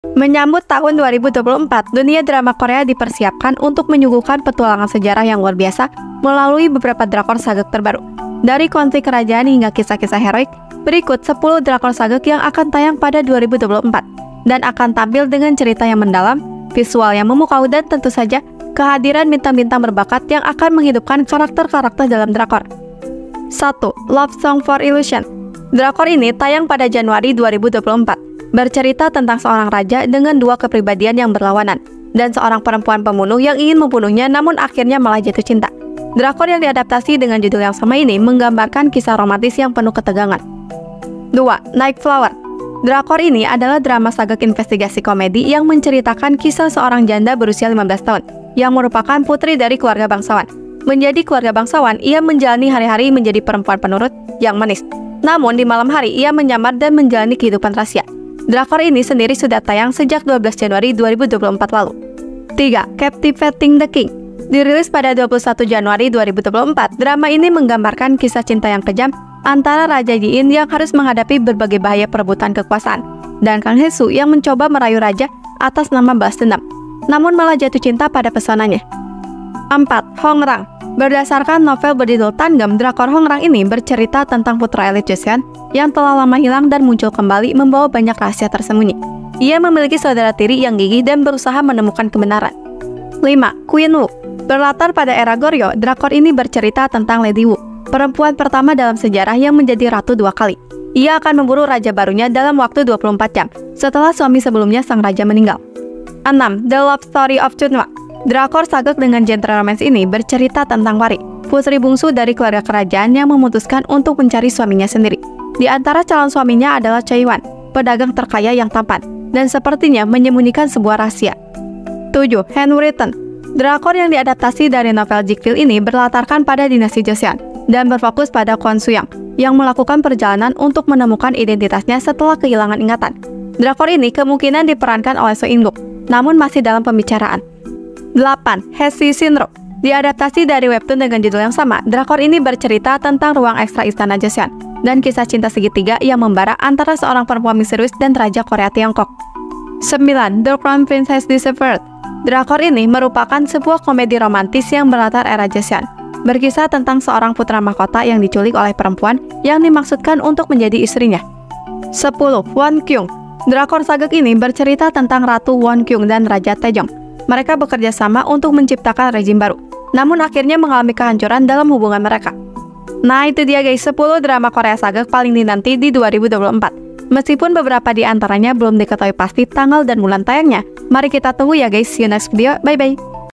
10 Drama Korea Sageuk Terbaru Ini Paling Dinanti di 2024, Ada yang Tayang di Netflix!
Menyambut tahun 2024, dunia drama Korea dipersiapkan untuk menyuguhkan petualangan sejarah yang luar biasa melalui beberapa drakor sageuk terbaru.

Dari konflik kerajaan hingga kisah-kisah heroik, berikut sepuluh drakor sageuk yang akan tayang pada 2024, dan akan tampil dengan cerita yang mendalam, visual yang memukau, dan tentu saja, kehadiran bintang-bintang berbakat yang akan menghidupkan karakter-karakter dalam drakor.